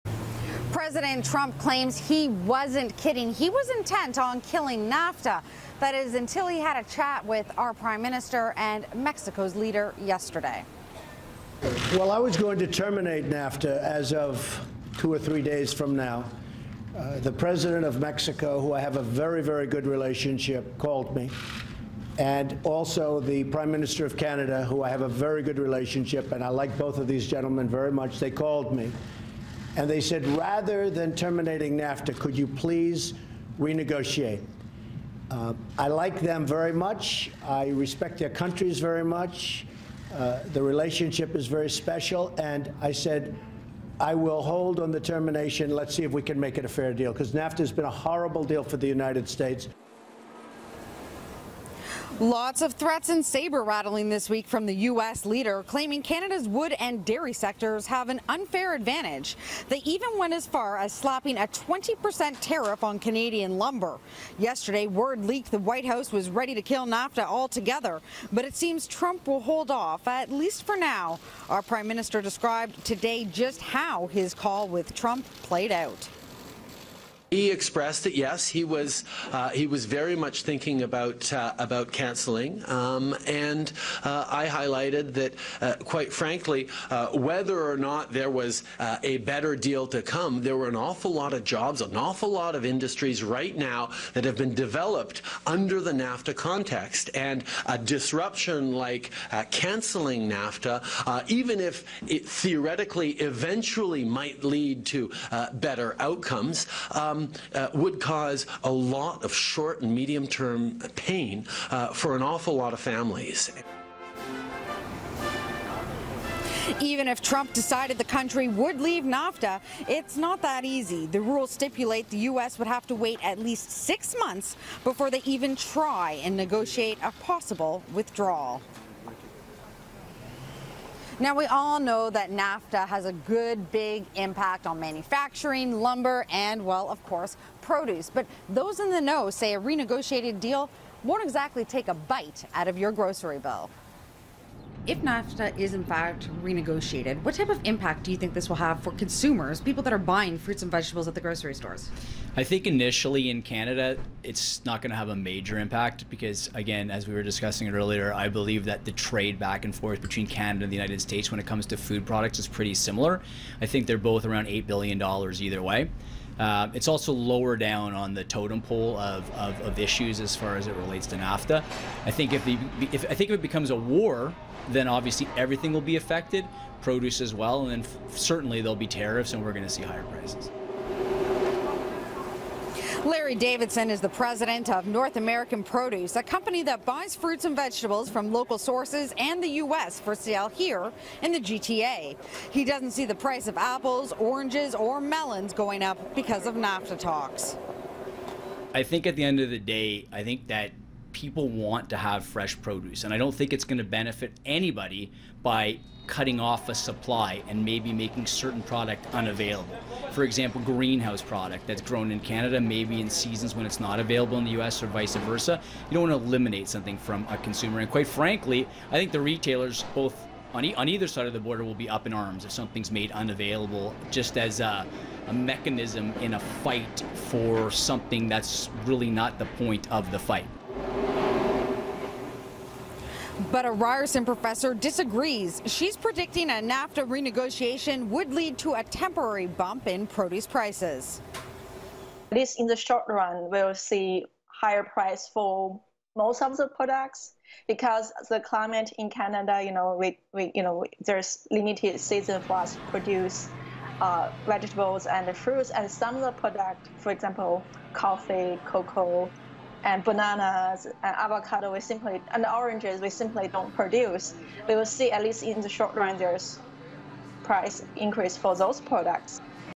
President Trump says he won’t pull out of NAFTA
President Donald Trump says he won't pull the U.S. out of NAFTA, but he is looking to renegotiate the free trade agreement. Reporter Cristina Howorun explores how this could affect us in Canada.